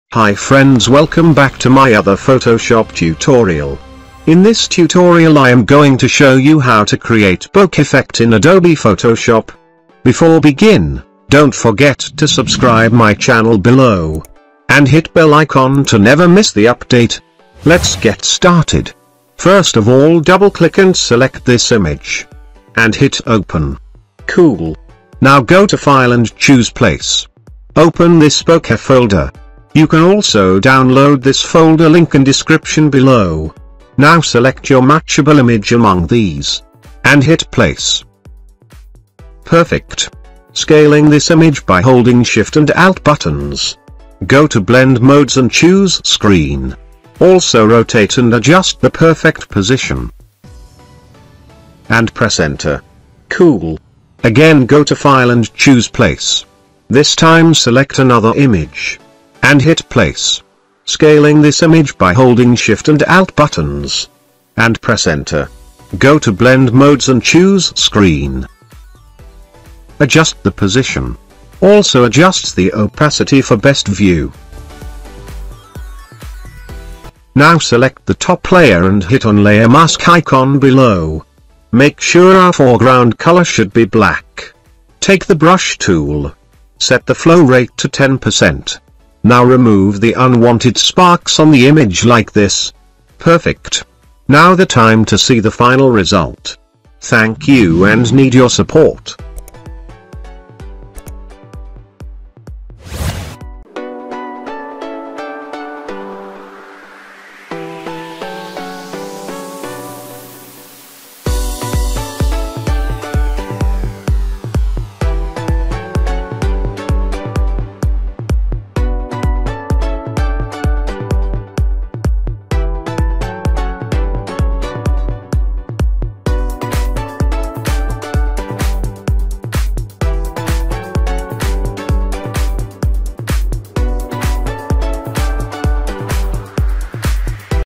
BOKEH Effect in Photoshop - 1 Minute ( Photoshop Tutorial )
Hi Friends, welcome back to other photoshop tutorial,
In this tutorial, you should learn how to create Bokeh Effect in Photoshop.

Click Here to View Channel: shorturl.at/deqrN

thanks to Photo By: Photo by Wallace Chuck from Pexels